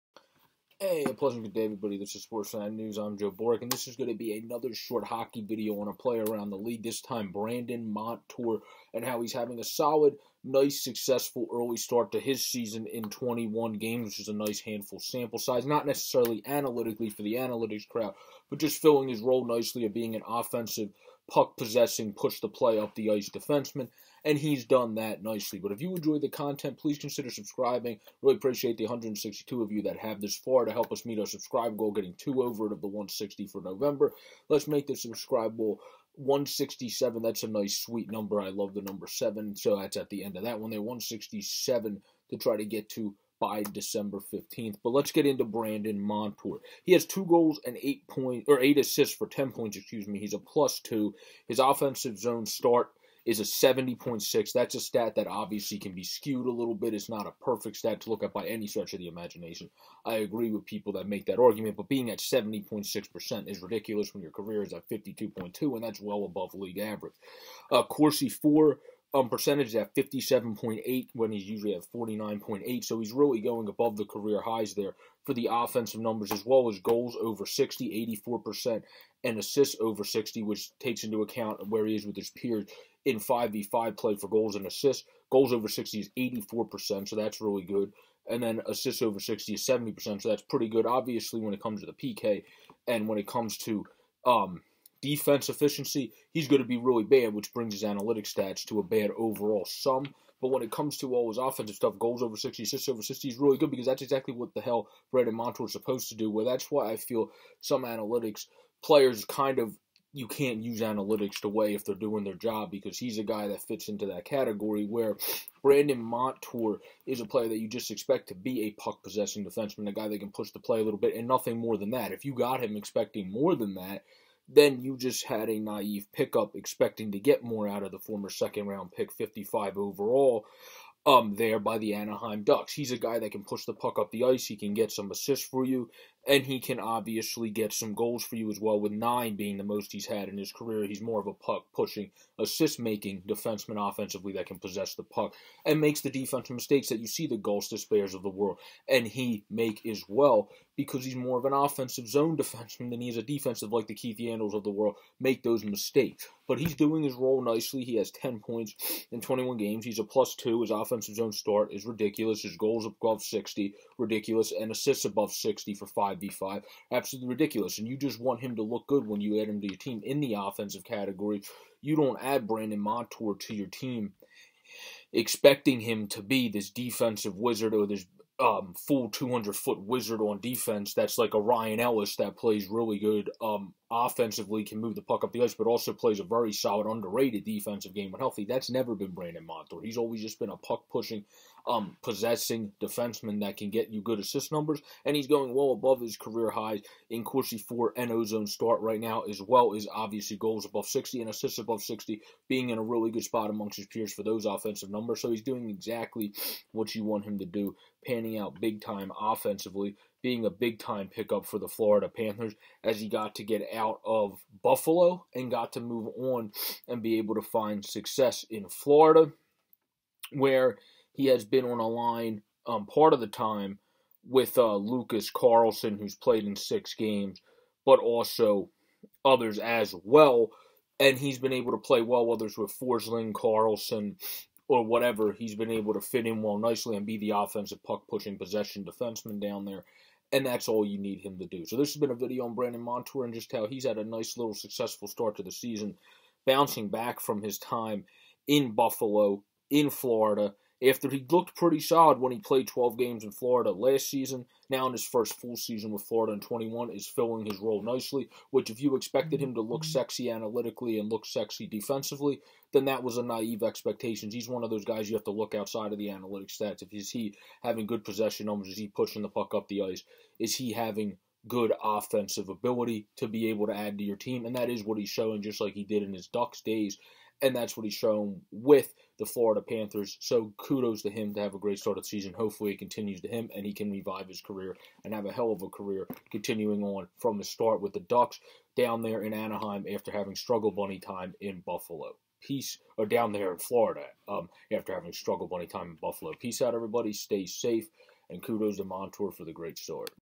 Brandon Montour having a Nice Offensive Start filling his Role for the Panthers
After bringing him in last season and playing 12 games Brandon Montour is now in his first full season with the Florida Panthers where he is filling his role of an offensive puck possessing defenseman well.  He is not a clean overall look analytically but is good there as well in some key 5v5 offensive categories which is Montour's bread and butter providing an offensive push from the back end. In this one Pro joe talks about that plus more as he highlights how Montour has filled his role nicely for the Panthers thus far and hopefully continues for him reviving his career after his stay in Buffalo. If you enjoy the content and have not already please consider subbing to keep us growing. Appreciate yall. Montour continues to prove to be a good pickup by the Panthers.